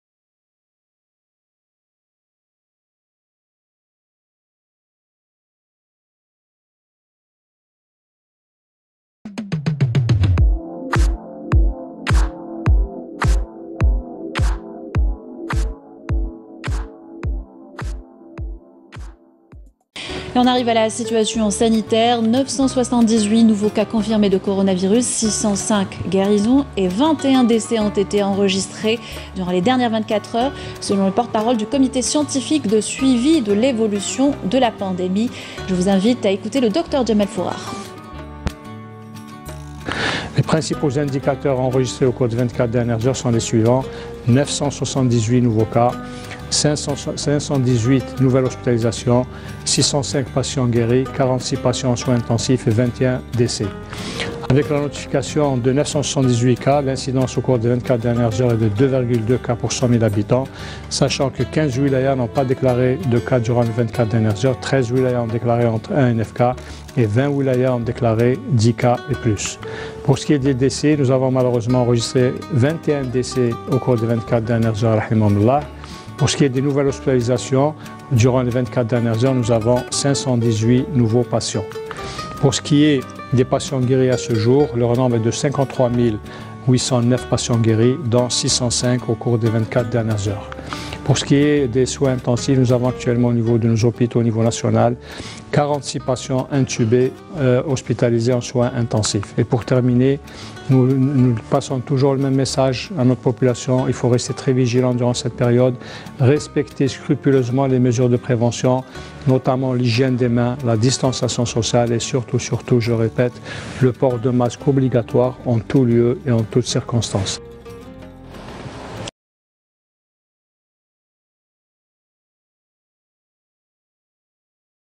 CORONAVIRUS ALGERIE Bilan du Lundi 30/11/2020
Neuf-cent soixante dix-huit (978) nouveaux cas confirmés de Coronavirus, 605 guérisons et 21 décès ont été enregistrés durant les dernières 24 heures en Algérie, a annoncé, lundi à Alger, le Porte-parole du Comité scientifique de suivi de l'évolution de la pandémie du Coronavirus, Dr Djamel Fourar.